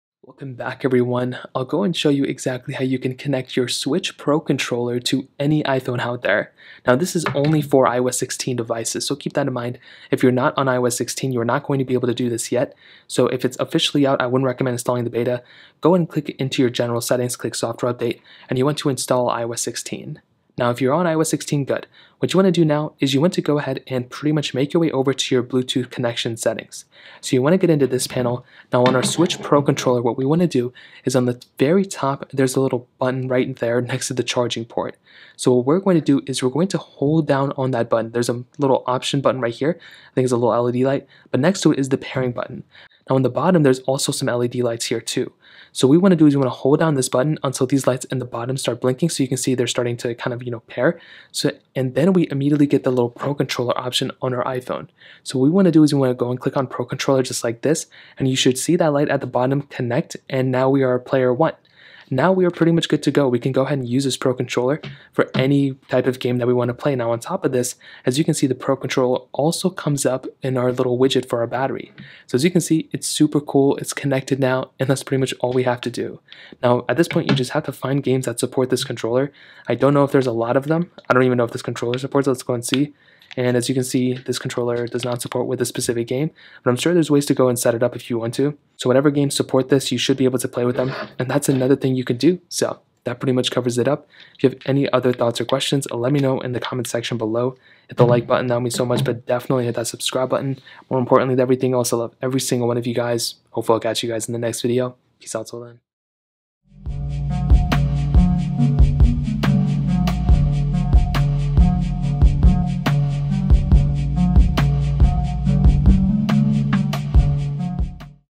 How To Connect Nintendo Switch Pro Controller To iPhone!
Here is exactly How To Connect Nintendo Switch Pro Controller To iPhone!